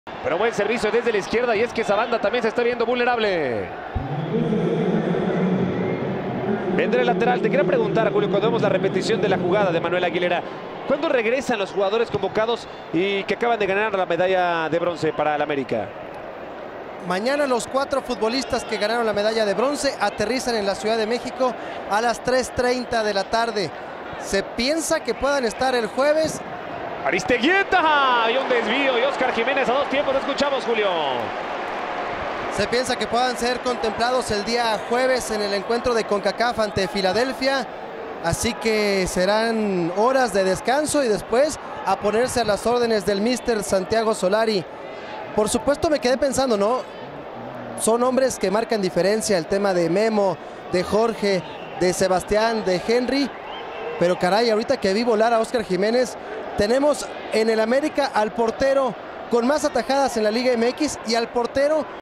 ¡Barrida salvadora! Aguilera salva al Ame | América 0-0 Puebla | Grita México BBVA AP2021 -J3 | TUDN
Pase filtrado que Miguel Layún corta de forma milimétrica.